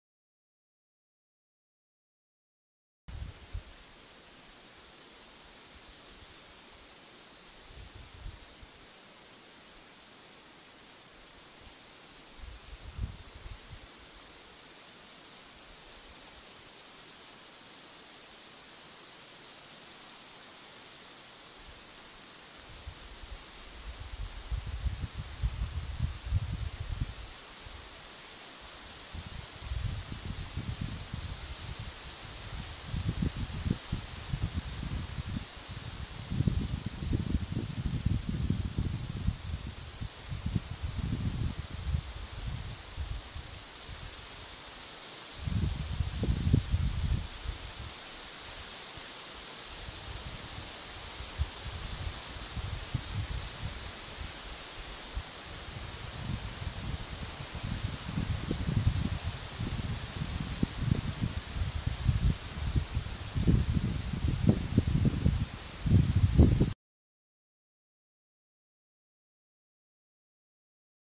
Brutalny zalotnik
Na nic się zdały długie, powłóczyste spojrzenia byka. Co tu jednak zrobić, gdy partnerka śpi, a temu na miłość się nagle zebrało? Gdy efekt „maślanych oczu” zupełnie nie zadziałał, adorator przeszedł do bardziej radykalnych metod budzenia.

Niniejszy materiał został opublikowany dzięki dofinansowaniu Narodowego Funduszu Ochrony Środowiska i Gospodarki Wodnej. Za jego treść odpowiada wyłącznie Fundacja Niezależne Media.